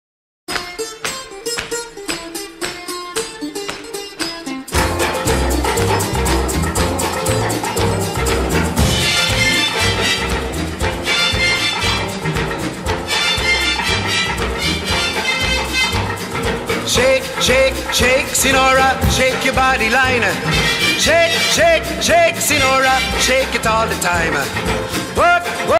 Belefonte Rumble
My son Alek put this together at the beginning of a stop-motion class. He is getting better and better :)